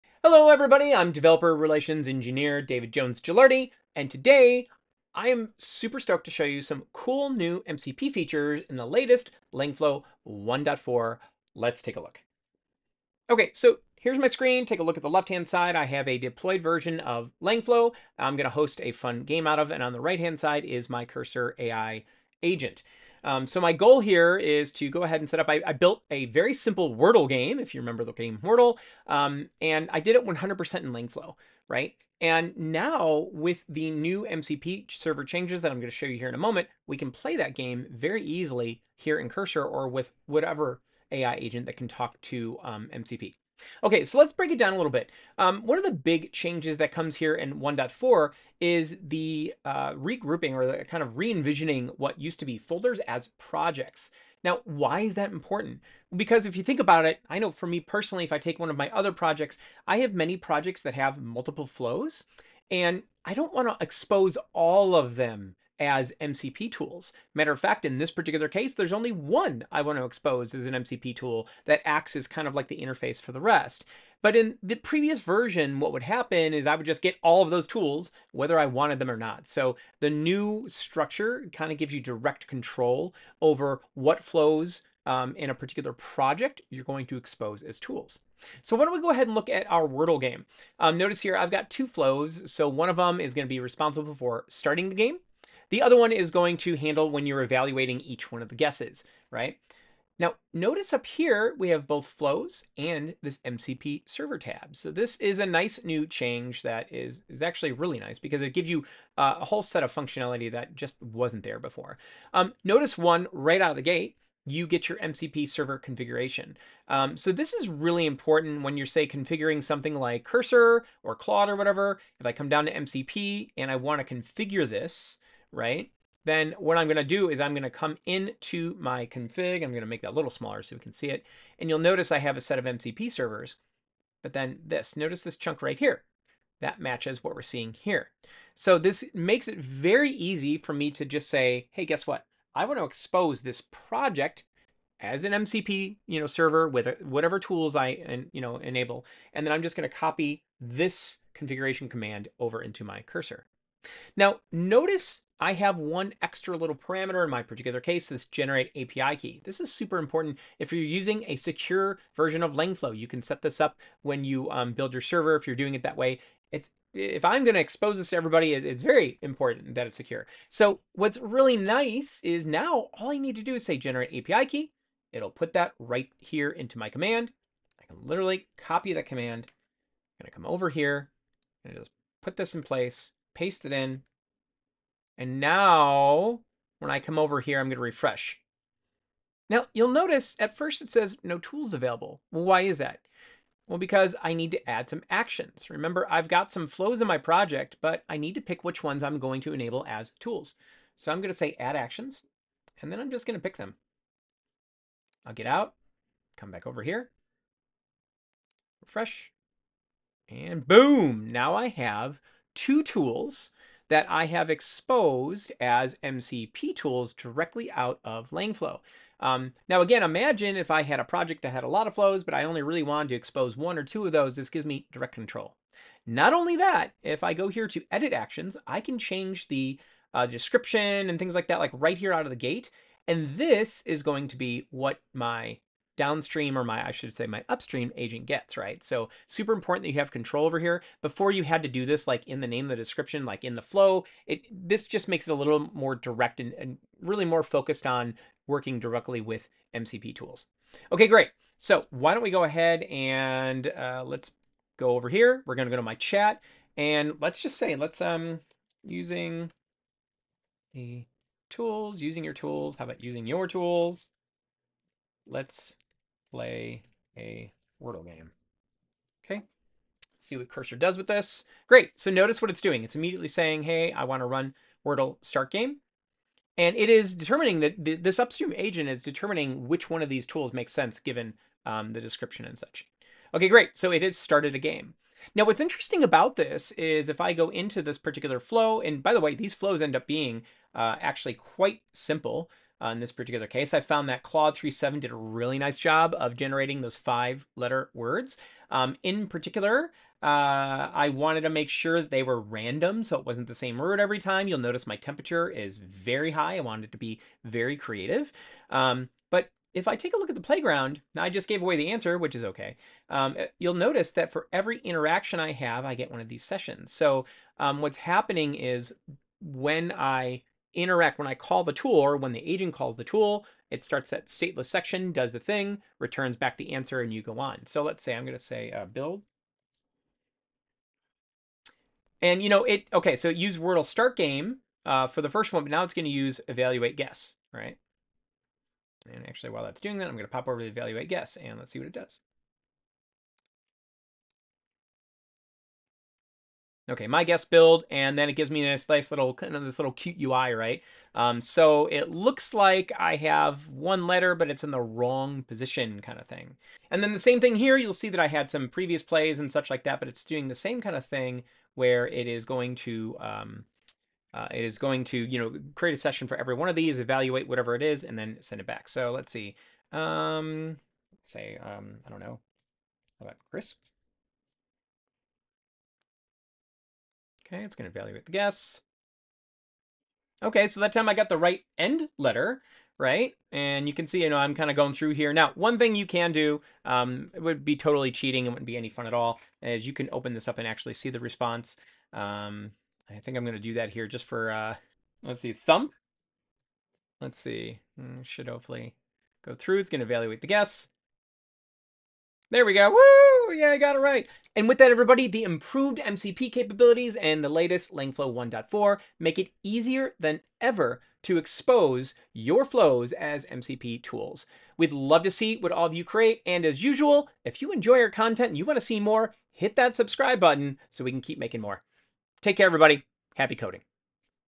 🧠 Build Agentic Games in MINUTES with MCP, Cursor, and Langflow 1.4
Langflow 1.4 is here—and it’s packed with powerful new capabilities for organizing, scaling, and exposing your AI workflows using MCP (Model Context Protocol).

In this demo, David shows how to use Langflow Projects to expose only the tools you want, generate secure MCP endpoints, and connect to apps like @cursor_ai. And yes—we play a fully agentic Wordle game powered entirely by Langflow & MCP!

Perfect for developers looking to modularize flows, wire up external tools, and supercharge agent collaboration. 💪

👇 What You’ll Learn:
🔹 What’s new in Langflow 1.4: Projects & MCP updates
🔌 How to expose flows as tools via MCP
🧩 How to control which flows appear in your MCP server config
🧠 How to connect Langflow to Claude Desktop, Cursor & more
🎮 How to build a Wordle game entirely over MCP!

⏱ Chapters:
00:00 – Intro: Langflow 1.4 Overview
00:45 – Use Langflow Projects to organize workflows for MCP
01:30 – Build an agentic Wordle game with MCP
01:59 – Expose a project as an MCP server
03:15 – Expose agent tools from Langflow workflows
04:28 – Wordle game agentic workflow
05:20 – Use Sessions in Langflow

Happy coding! 👋